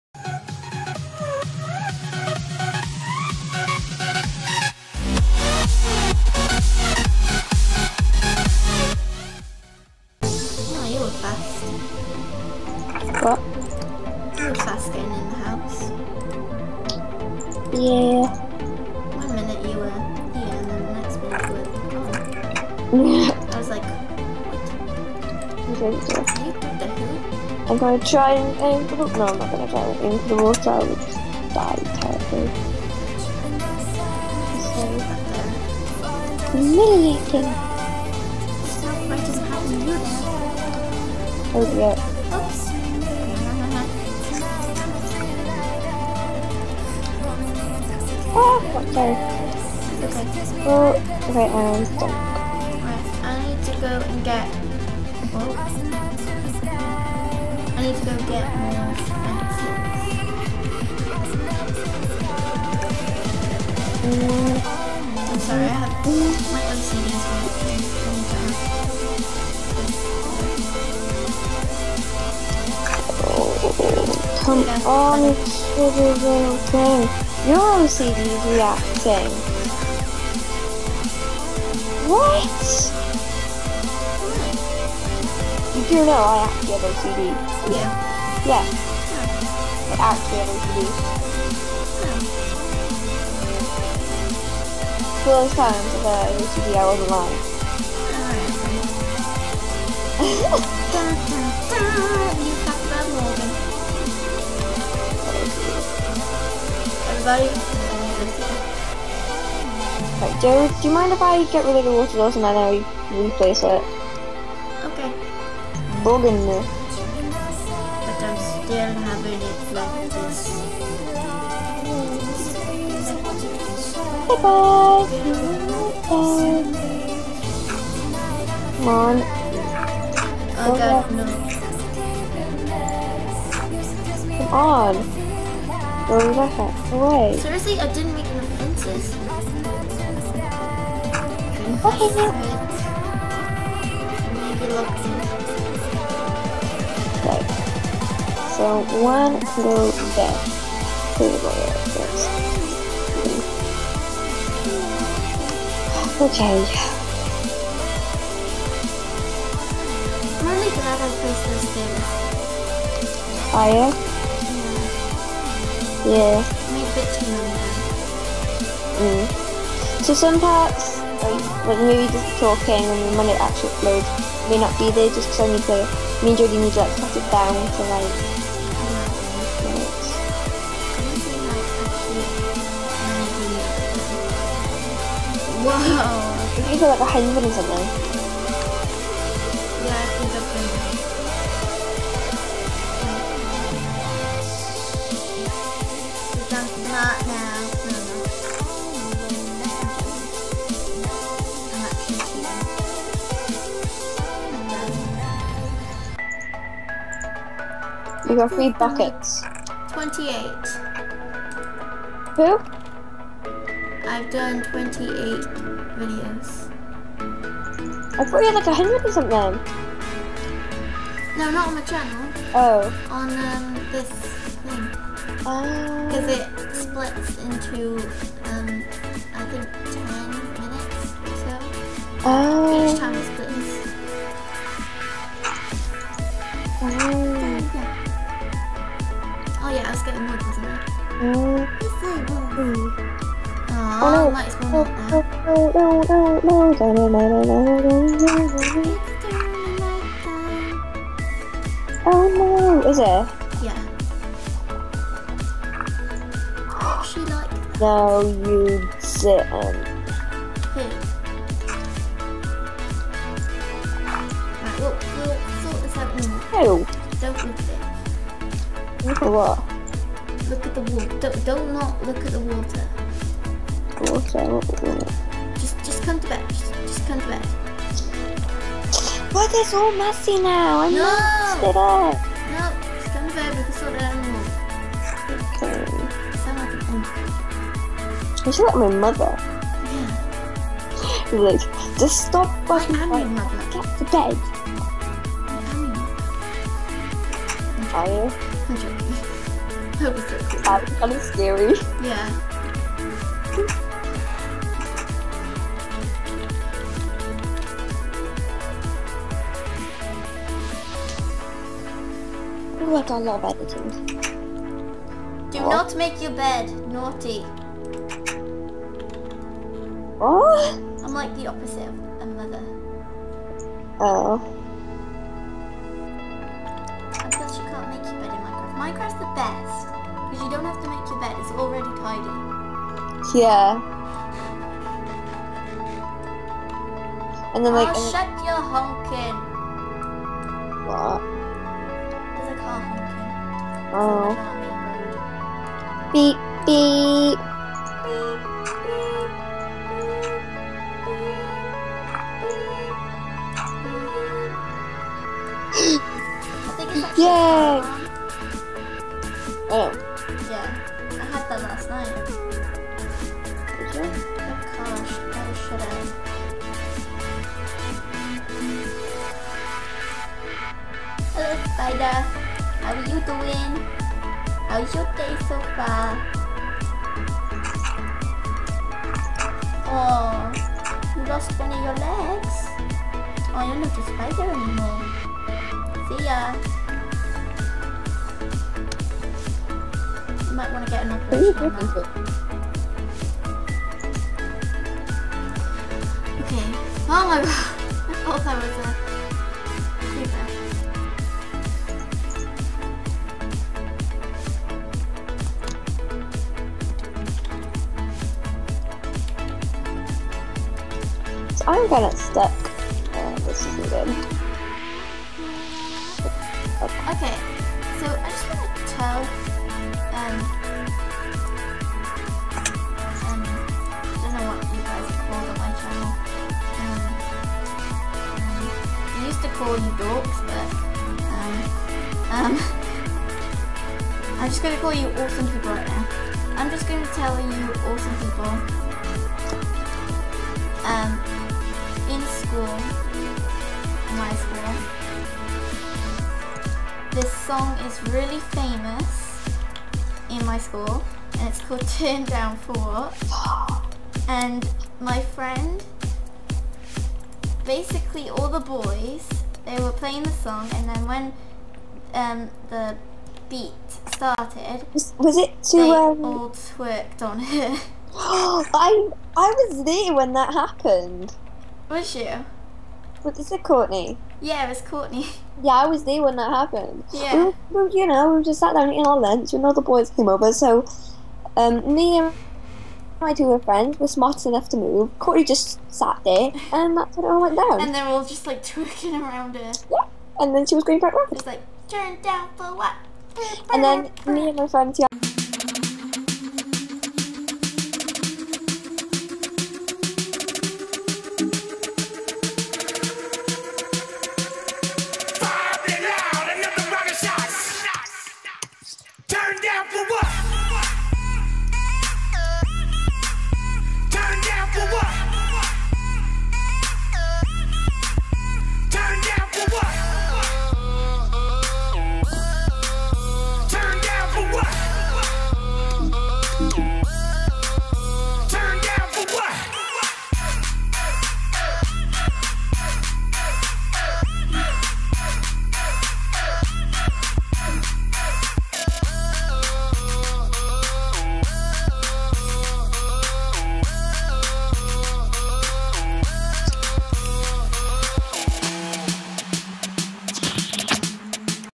Yognaughts Re-Unite/w BlueFlowerGaming xD ep5
Here is mine and Blue's new series on our own private server, this is a vaniller series where we will go to the end and make an inspired world by Stampylongnose. so hope you like this new series :D seeya my joey's ;)